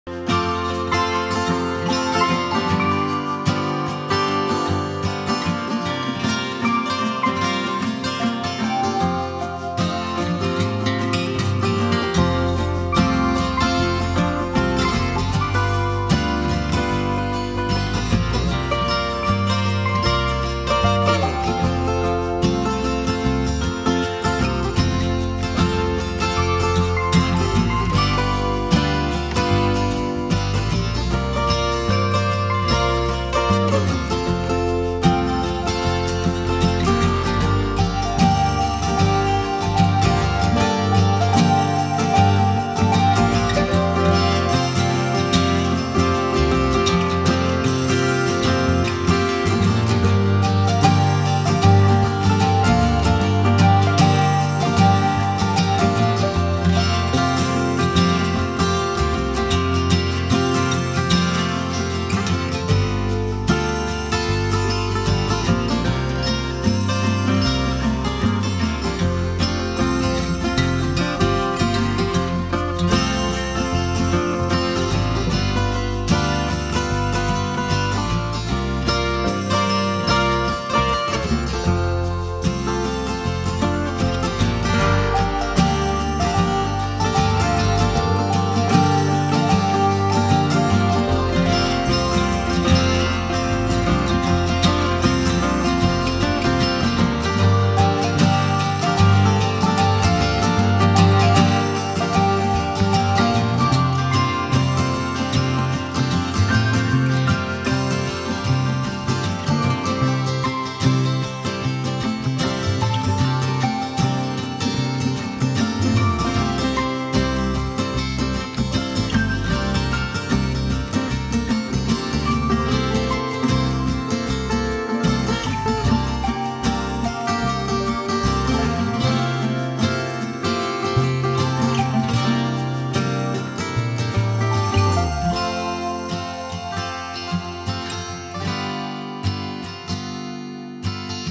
Old schoolhouse off 400 highway in southeast Kansas.
I was cruising down 400 east of Wichita and found this old schoolhouse on the south side out in the middle of a field. Very cool old building, made a great little video shoot.